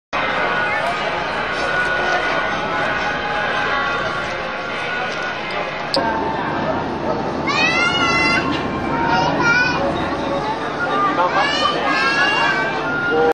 路面電車まつり（広島）Hiroshima tram festival 6
マスコットの「電車くん」が車庫内を歩く。tram mascot "Densha-kun"